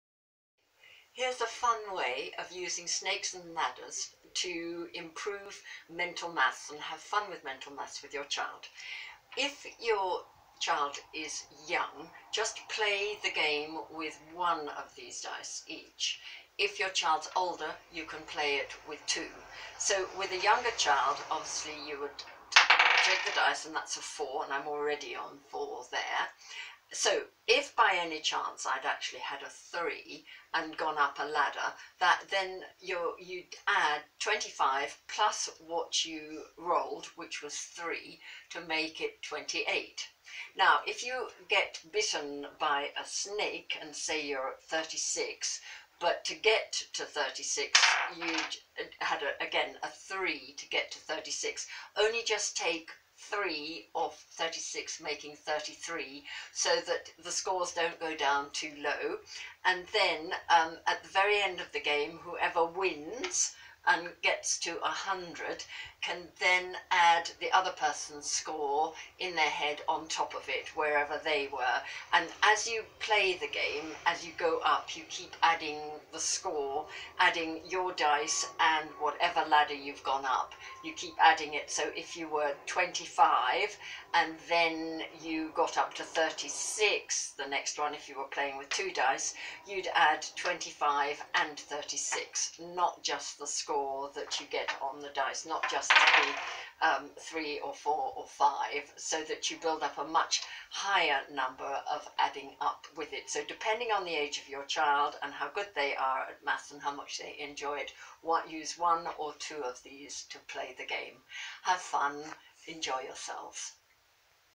Fun Mental Math with "Snakes and Ladders"
Most children love playing Snakes and Ladders, especially if they win. If you add mental math to the game and show your child how you add up when they can't do it,  they will enjoy learning.